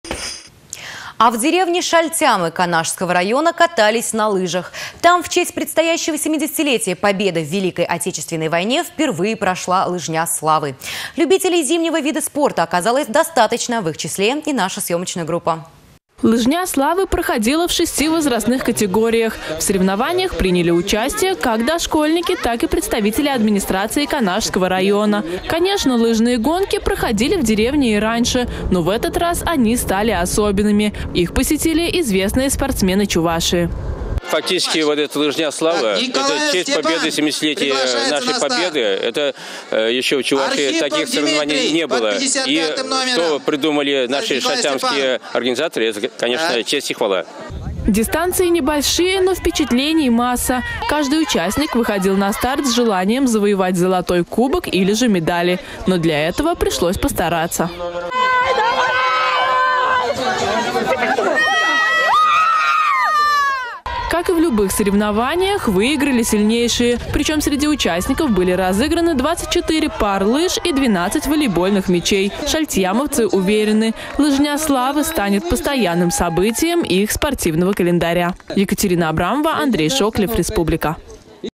В деревне Шальтямы Канашского района впервые прошла «Лыжня славы-2015»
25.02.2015 В этом году Россия празднует 70-летие со дня победы в Великой Отечественной войне. В честь этого события в деревне Шальтям Канашского района впервые прошла «Лыжня славы-2015». Любителей зимнего вида спорта оказалось достаточно, в их числе и наша съемочная группа.